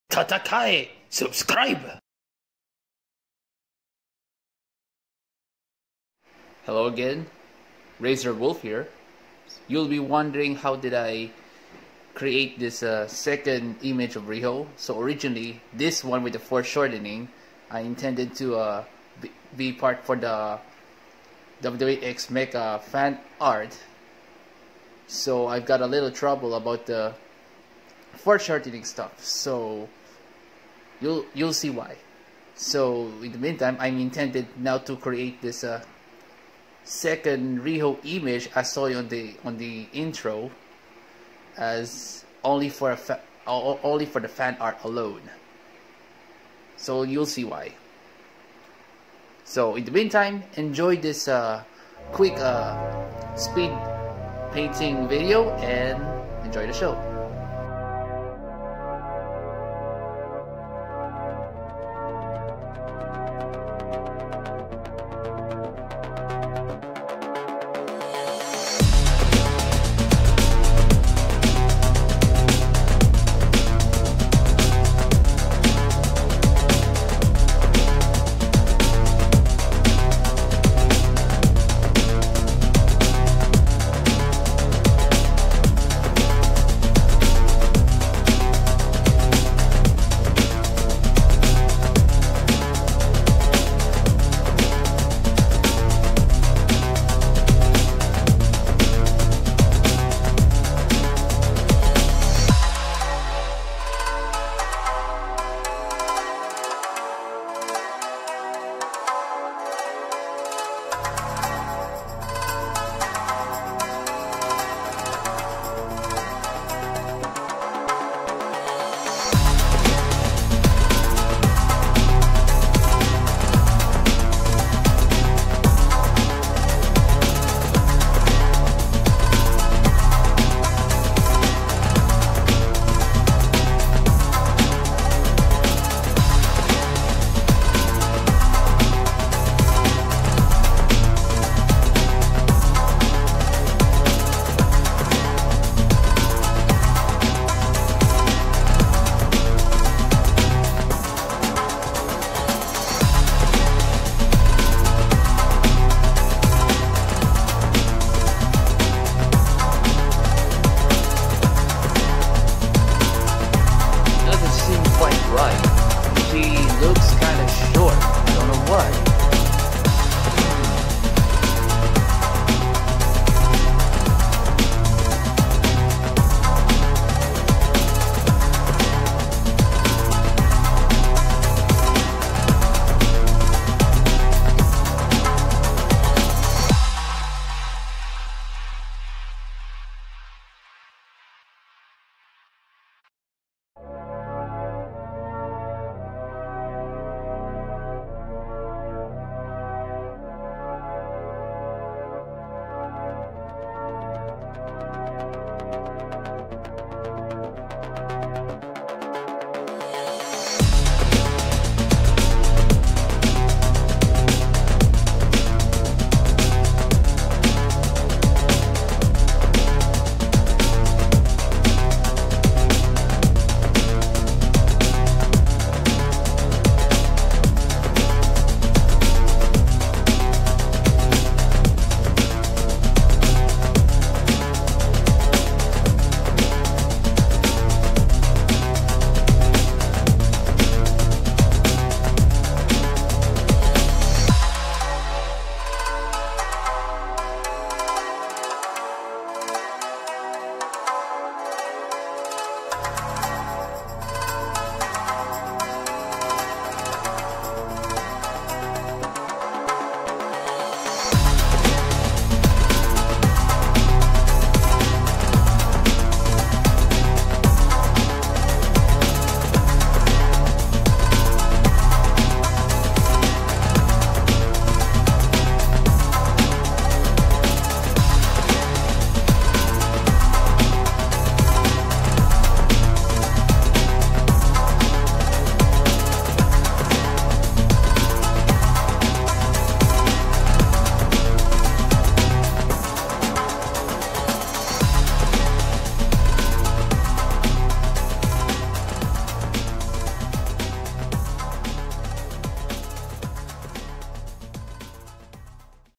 AEW Fanart: Riho
This speedpainting focuses on Riho fanart. I'm aiming not for perfection of this drawing/painting in this video but understanding more on foreshortening and fixing errors.

[DEVIANTART]
/tredecies-razorwolf

Support me for more exclusive WIPs and other content by joining membership on...

[ETH]
0x164Cee9B7B79e625b845fadD26CCbC83dA529d39

[XRP]
rQhbKHzKFXjSshqythFwNozznQPS1PVSQC

BGM